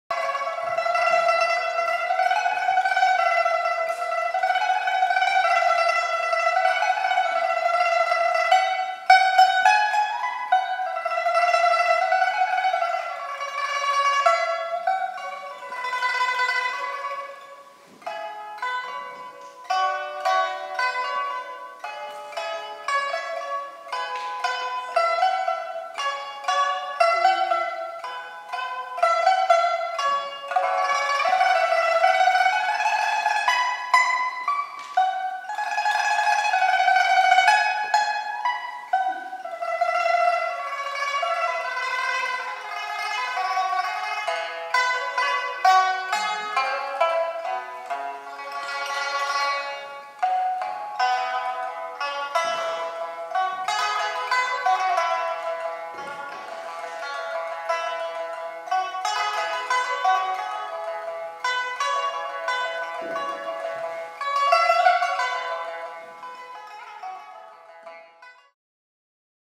The Ensemble of the Music Confucius Institute v Copenhagen World Music Festival 2015
Music Confucius Institute (MCI) blev etableret i 2012 efter et mangeårigt samarbejde mellem Central Conservatory of Music i Beijing of Det Kongelige Danske Musikkonservatorium i København. Instituttets formål er at facilitere musikalsk og kulturel udvikling og udveksling mellem Kina og Danmark.
Ensemblet består af guqin, guzheng, pipa, yangqin, og erhu, som er traditionelle kinesiske instrumenter.